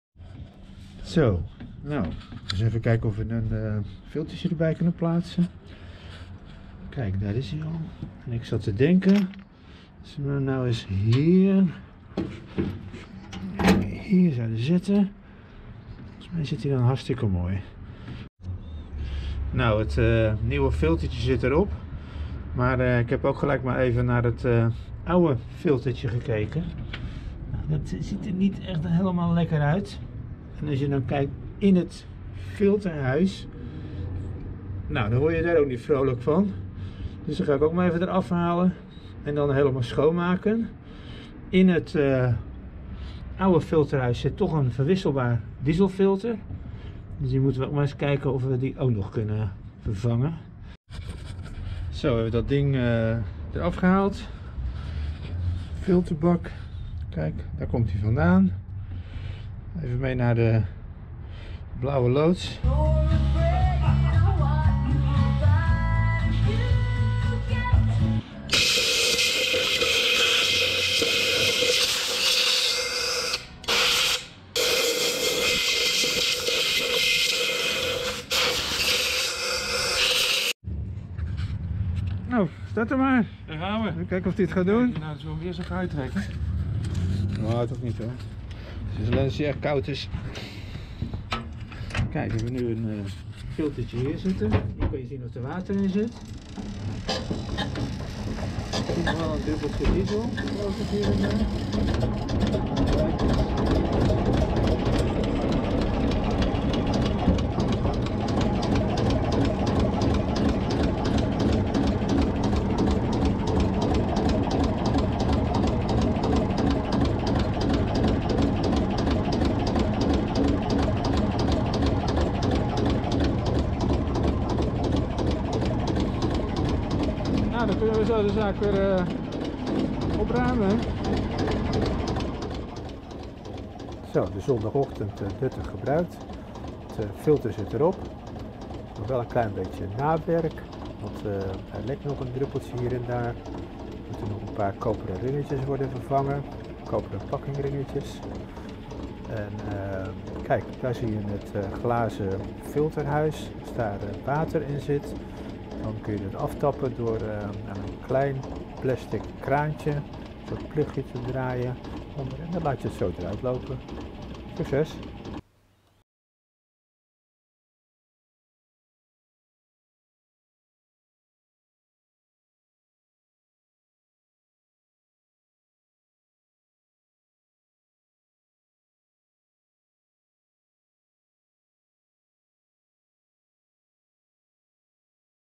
Extra filter GEIN
De laatste paar weken last gehad van water in de brandstof. Geeft veel problemen. Daarom extra filter geplaatst met waterafscheider. Als er water te zien is in de glazen pot onder het filter dan aftappen met behulp van het aftapplugje onder het glazen filterhuis.